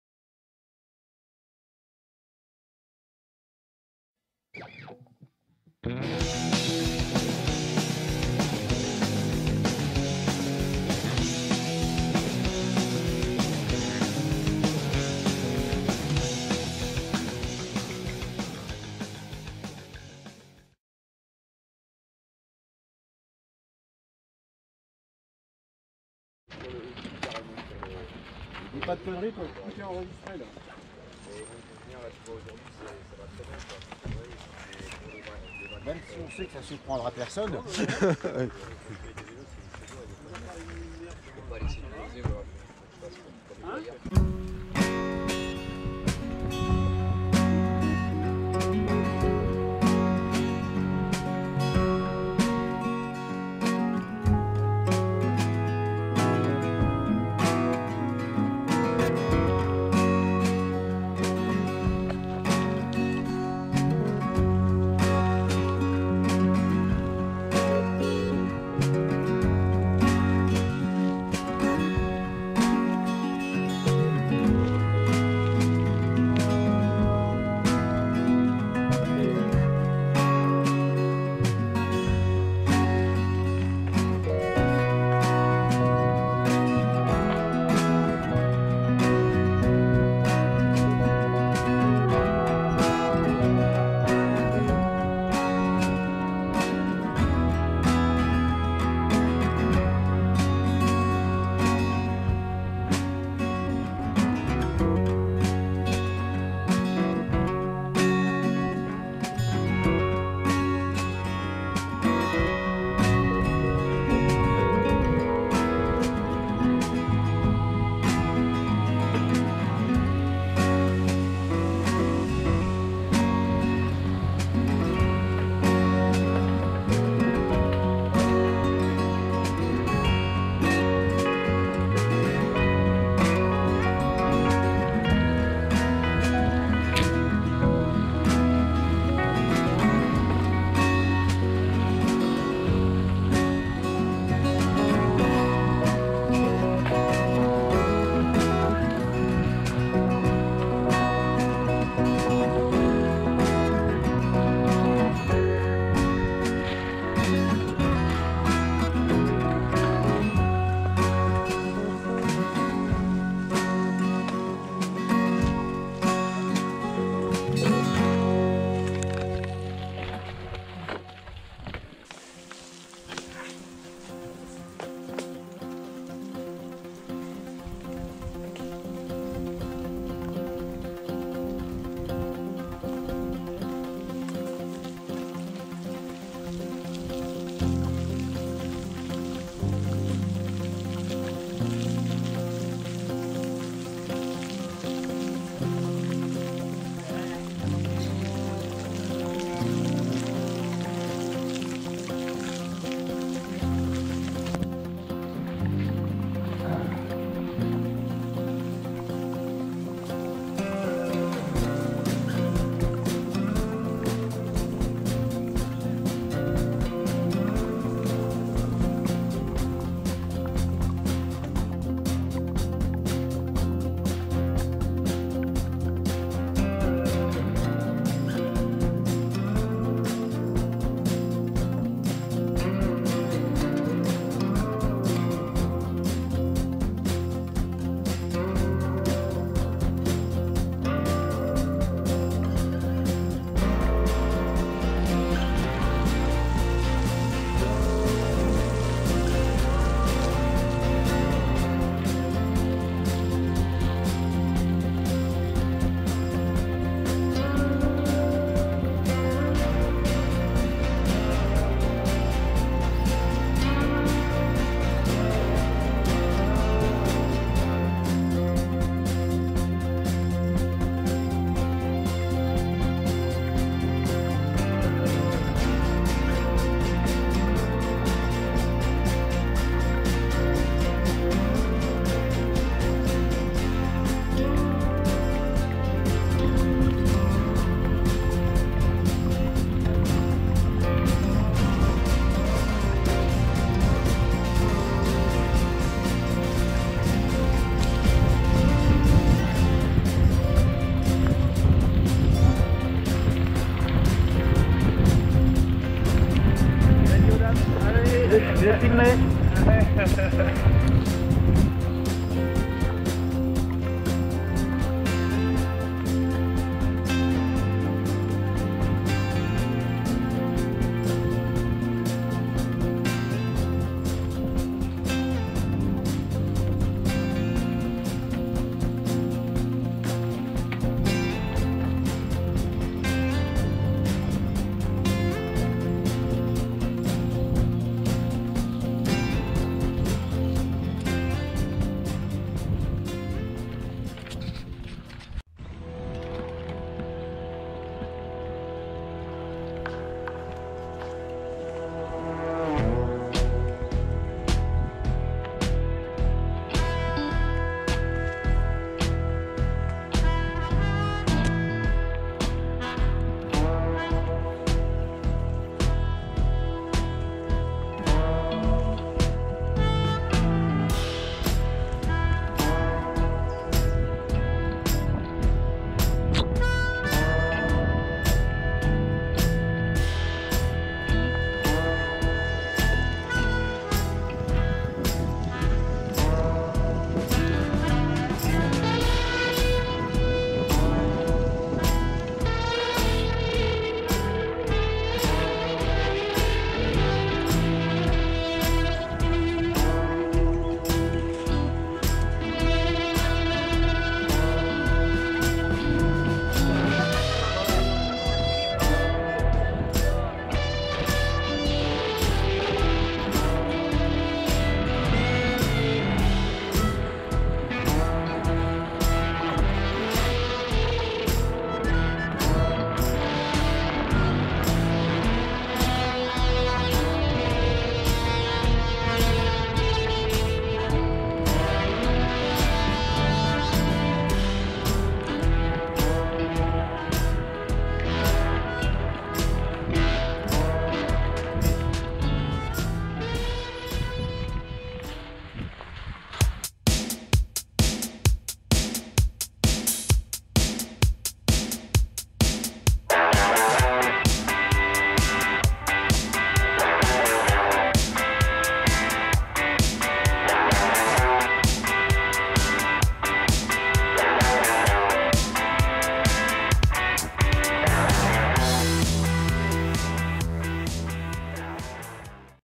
Maxiverte 2023 Samatan Vendredi
Le club de rando parcillon s'est déplacé en nombre dans le Gers pour participer à l'édition 2023 de la Maxiverte. Une Maxiverte un peu boueuse, surtout le vendredi, mais particulièrement bien organisée. Des ravitos quasi gastronomiques (foie gras, ventrèche, magret...) et un accueil exceptionnel. Un grand merci à l'équipe organisatrice.